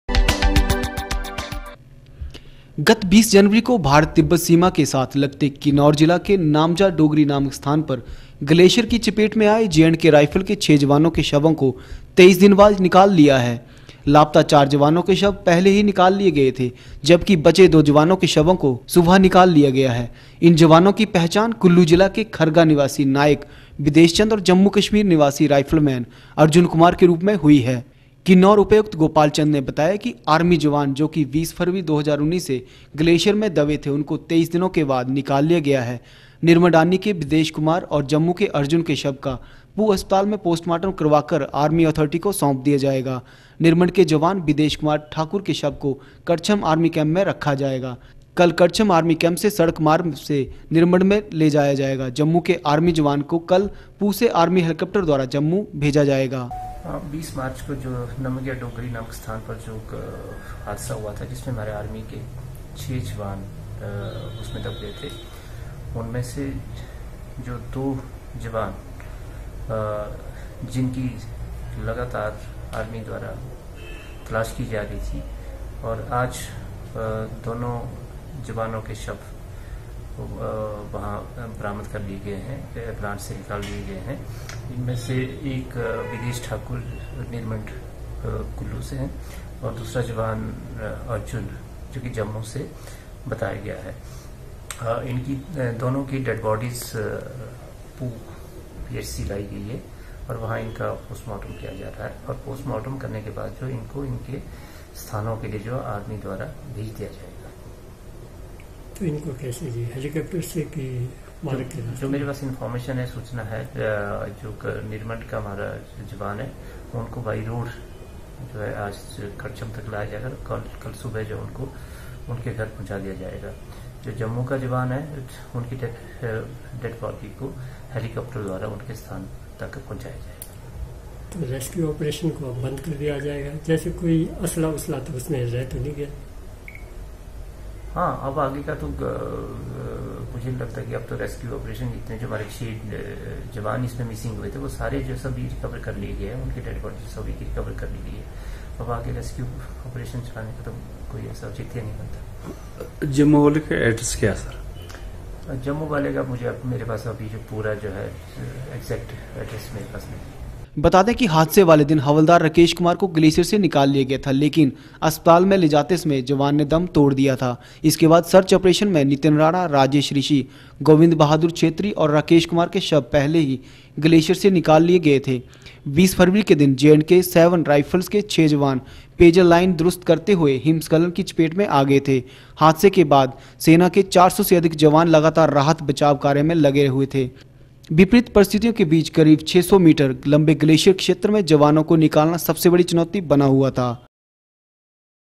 जे एंड के राइफल के 6 जवानों के शवों को निकाला गया 23 दिन बाद
गत 20 जनवरी को भारत-तिब्बत सीमा के साथ लगते किन्नौर जिला के नामज्ञा डोगरी नामक स्थान पर  ग्लेशियर की चपेट में आए जेएंडके राइफल के 6 जवानों के शवों को 23 दिन बाद निकाल लिया गया है। लापता 4 जवानों के शव पहले ही निकाल लिए गए थे जबकि बचे दो जवानों के शवों को गुरवार सुबह निकाल लिया गया है।  इन जवानों की पहचान कुल्लू ज़िला के खरगा निवासी नायक विदेश चंद और जम्मू कश्मीर निवासी रायफलमैन अर्जुन कुमार के रूप में हुई है।